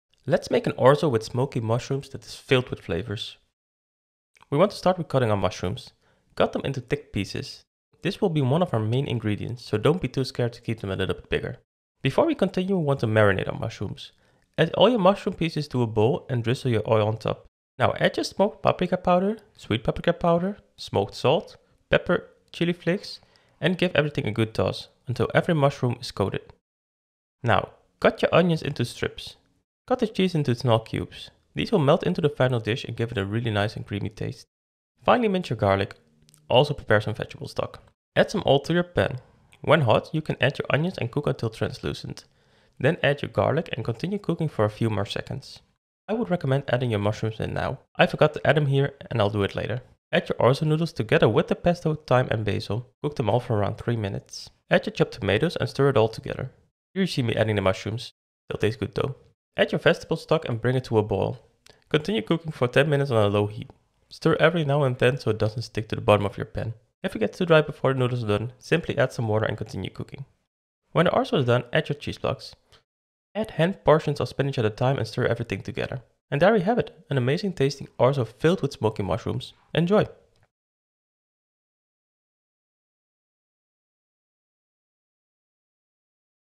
Orzo with Smokey Mushrooms
This orzo is filled with smokey mushrooms, it’s a tasty and very easy recipe!